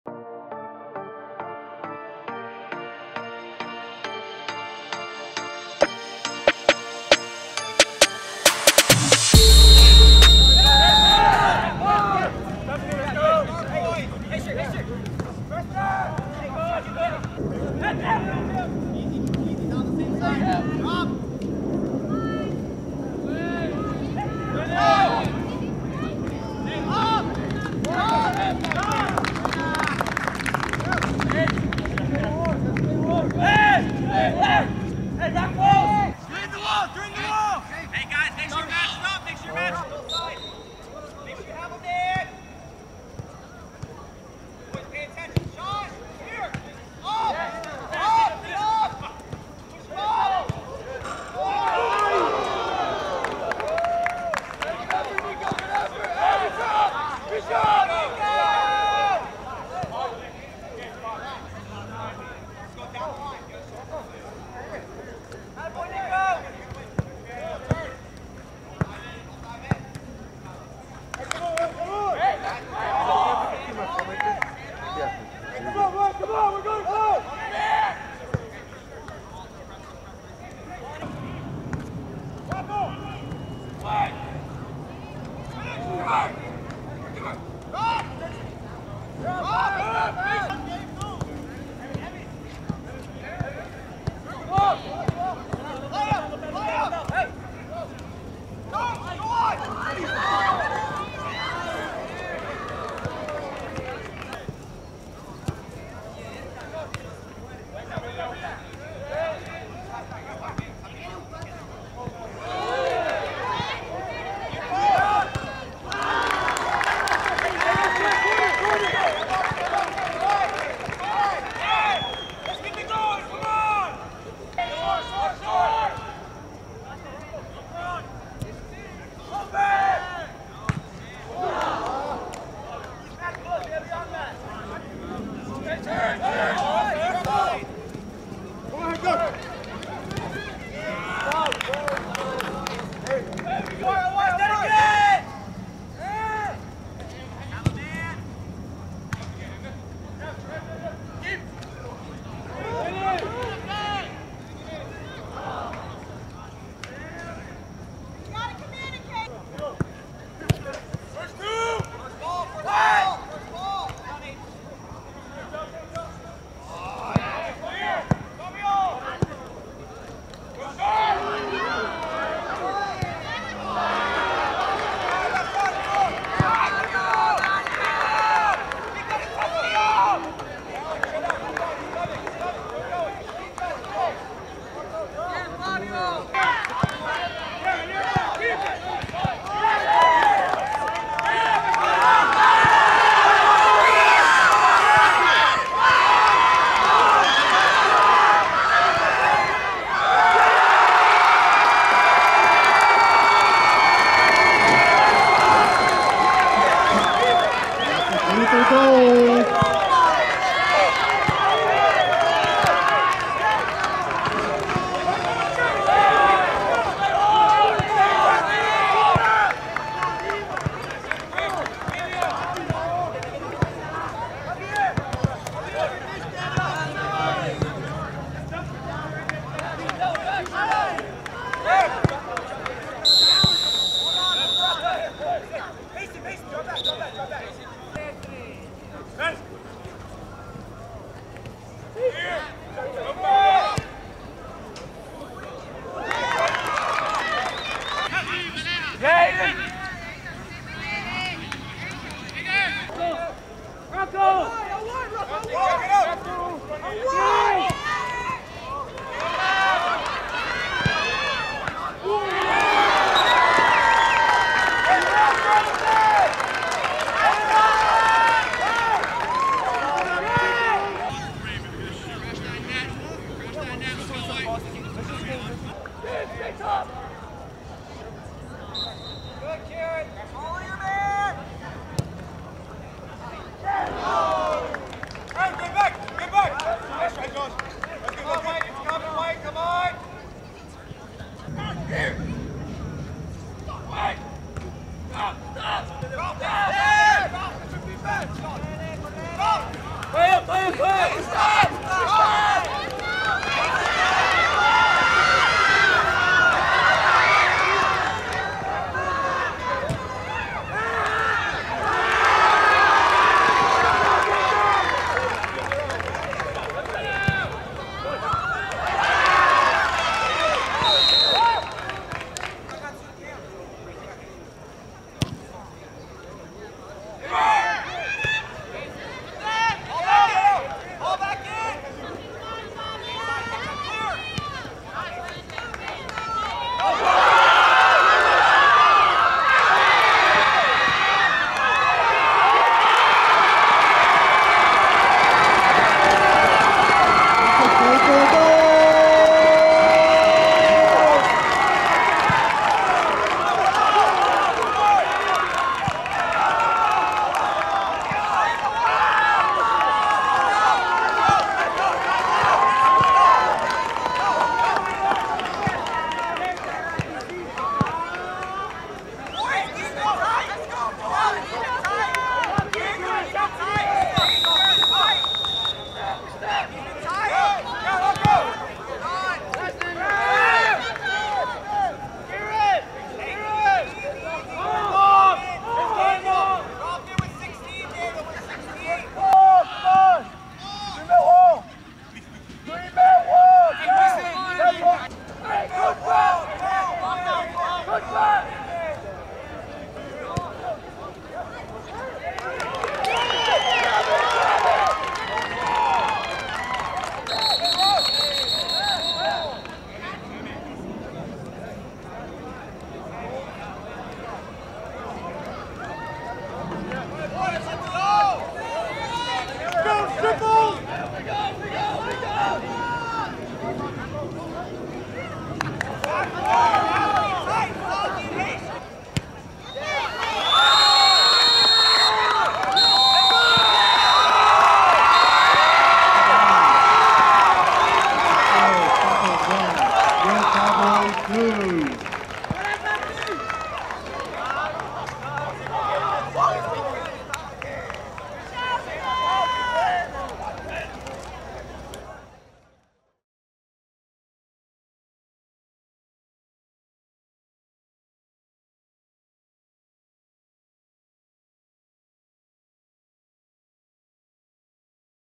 DRAMA IN FINAL MINUTES | Florida High School: Sickles High School vs Gaither High School | 11.17.23
Sickles High School vs Gaither High School | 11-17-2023
League: Florida High School
Venue: Gaither High School
Tampa, FL